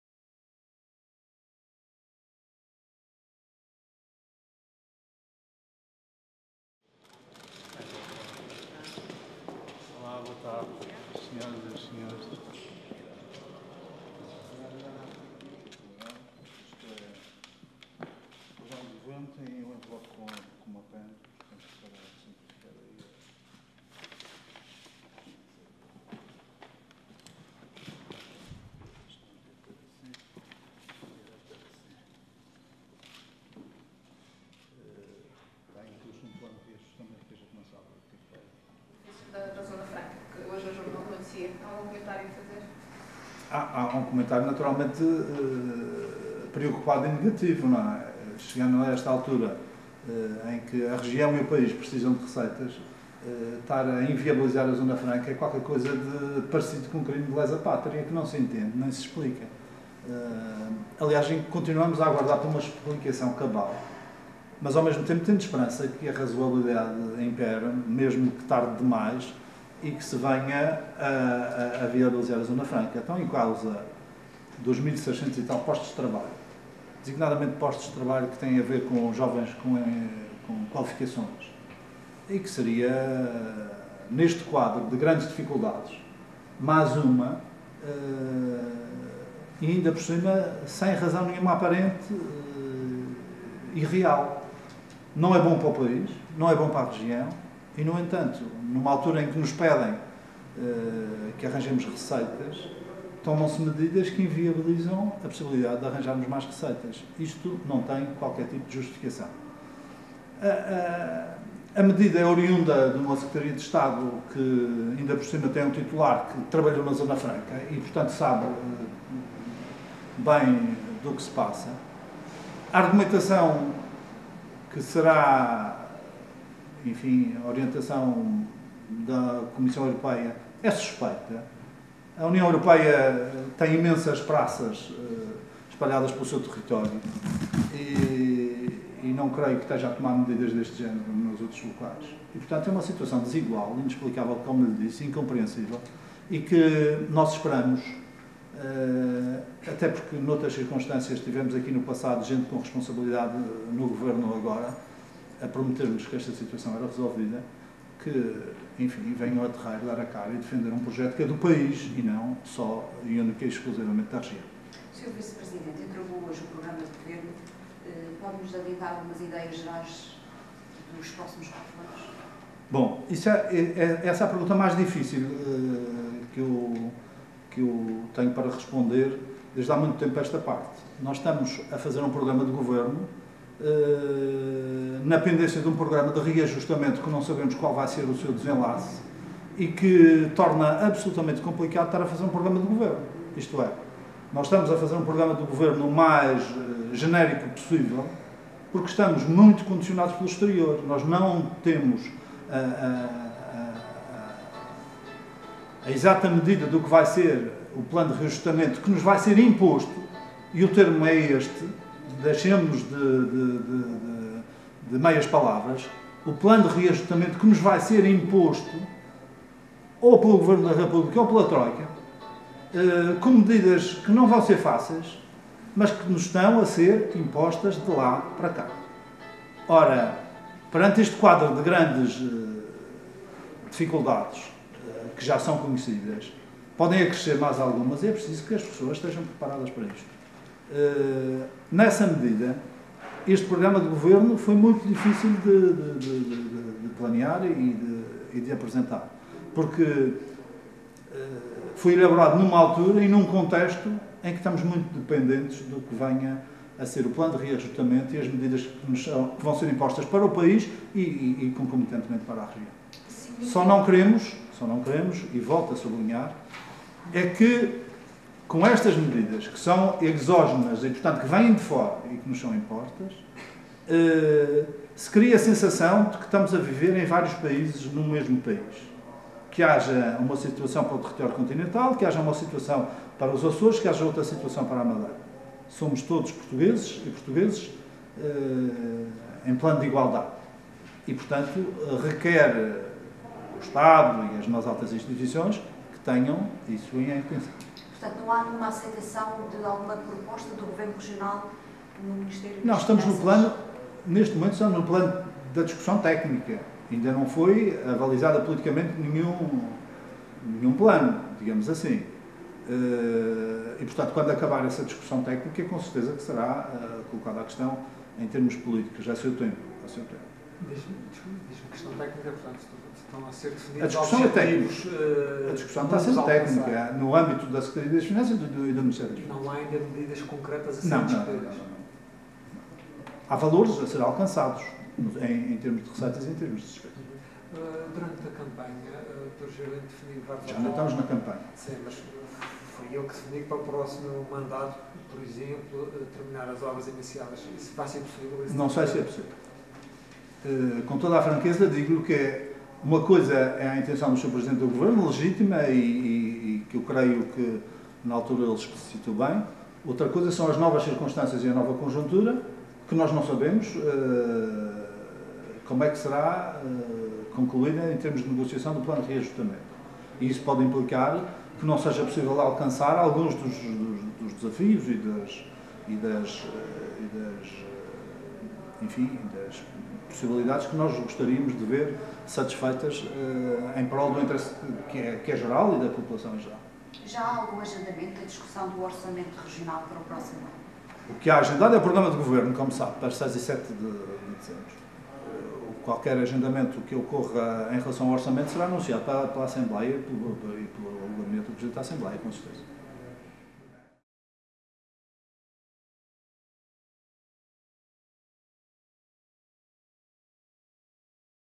2011/11/30 - Entrega do Programa de Governo na Assembleia Legislativa da Madeira
O Vice-Presidente do Governo Regional entregou o Programa de Governo para o quadriénio 2011/2015, na Assembleia Legislativa da Madeira